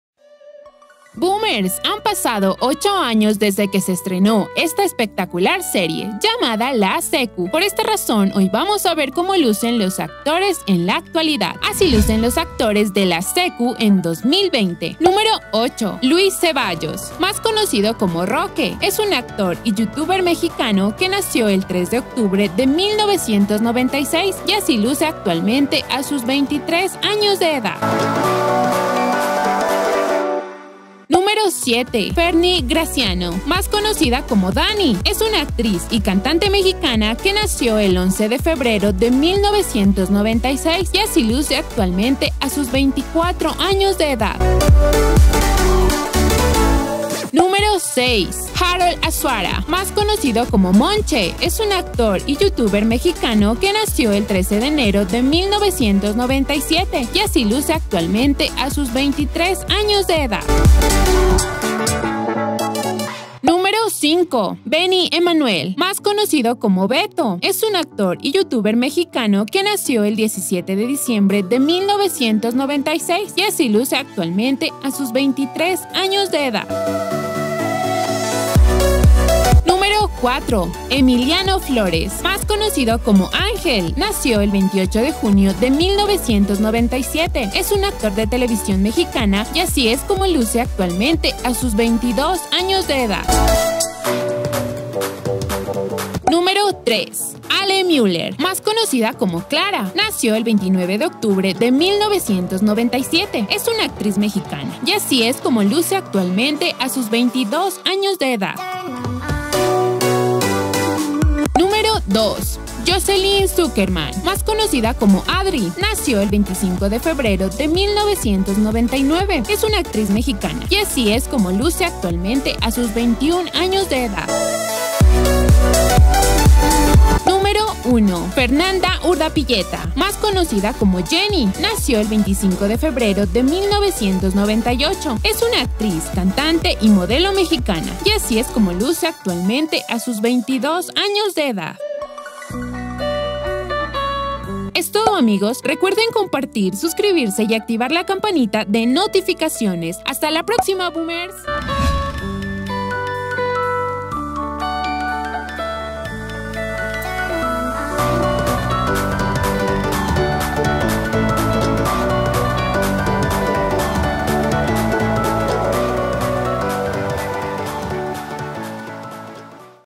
ASÍ LUCEN LOS ACTORES DE LA CQ EN 2020
Hola Boomers! Hoy Vamos a Ver Como Lucen Los Actores De LA CQ En [2021]

-LA CQ ANTES Y DESPUÉS 2020
-LA CQ BEFORE AND AFTER 2020
-LA CQ ANTES E DEPOIS 2020

HASTA LA PRÓXIMA BOOMERS!